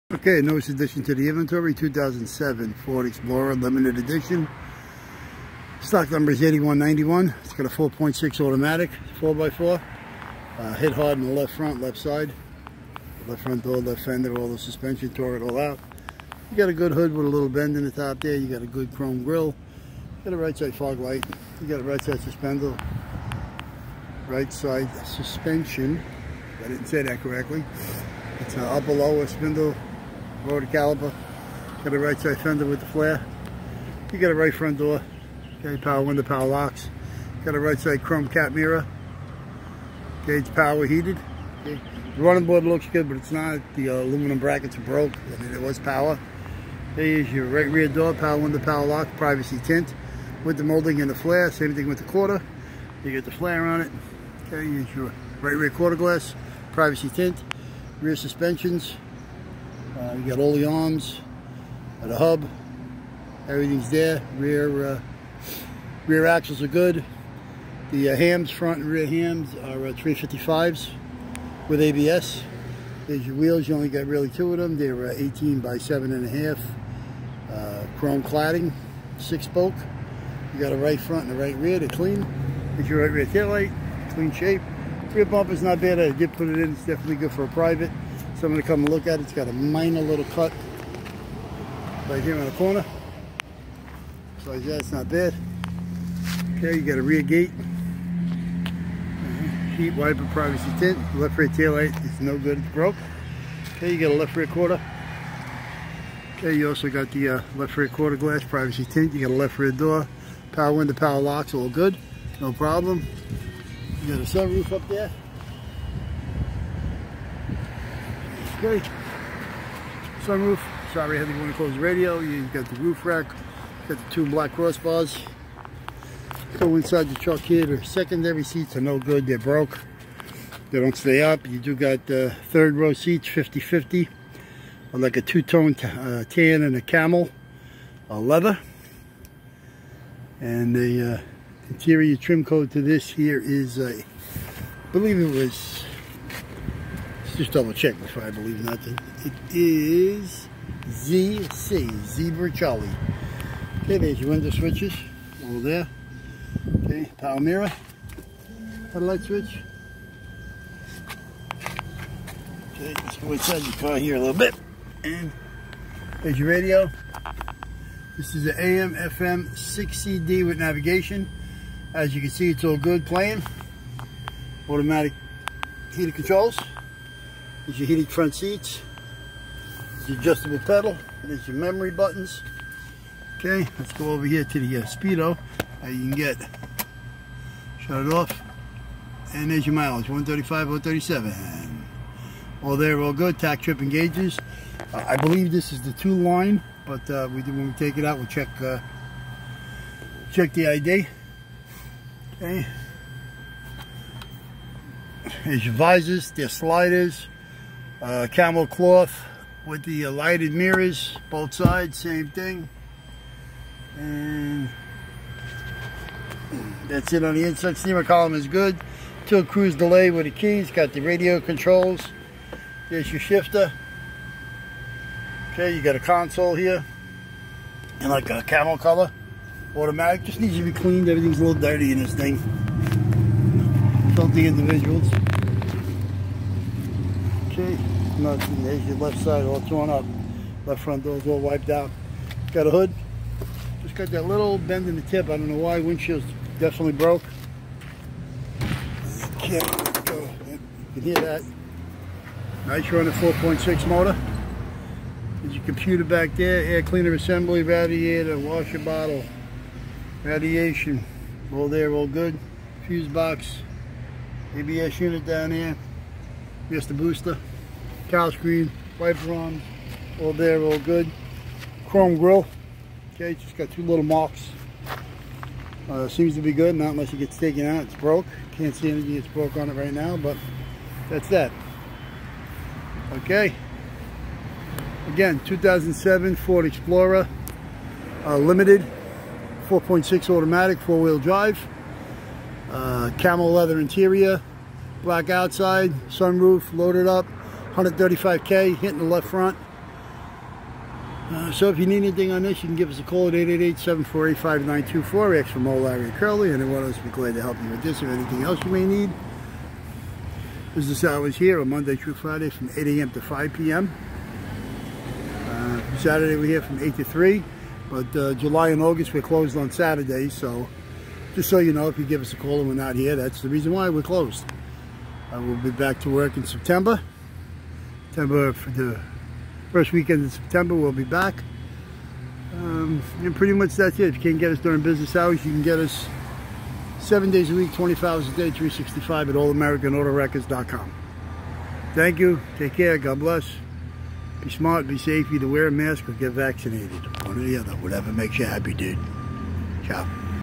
All American Auto Wreckers is Dismantling this 2007 Ford Explorer Limited 4.6 auto 4x4 Stock #8191
This 2007 Ford Explorer Limited Stock # 8191 was hit left front. This Explorer also has a good running 4.6 engine and a 4x4 automatic transmission.  The body is in good shape with most parts ready for sale. The Gear ratio in this 2007 Explorer is 3.55 non locker. This Explorer has abs brake system on all four wheels. We also have complete rear suspensions. And rear frame is also good. The Exterior Paint Code is UA-Black and the Interior trim Code-ZC Camel. The Build Date is 1/07. So if Moe-Larry or curly can help you with any parts from this 2007 Ford Explorer Limited.